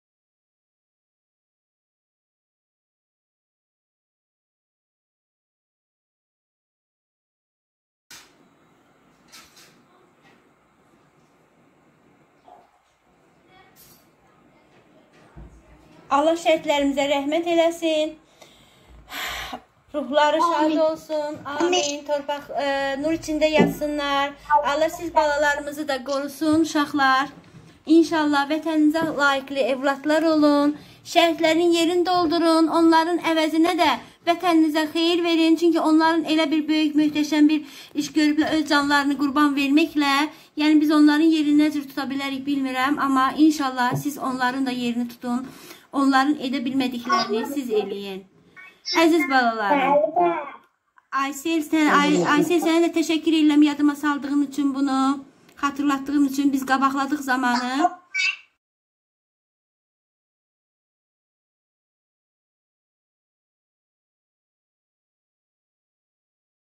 Şəhidlərin ruhuna dəqiqəlik Anım Sükutu 4N-li Xankəndi Məktəbi 4a sinifi. İngilis dili dersi
Şuşa rayonu Nizamı Gəncəvi adına Xankəndi şəhər 4N-li tam orta məktəbin 4a sinif şagirdləri ingilis dili dərsində Şəhidlərimiz ruhunu 1 dəqiqəlik sükutla yad etdilər.
Allah Şəhidlərimizə rəhmət eləsin!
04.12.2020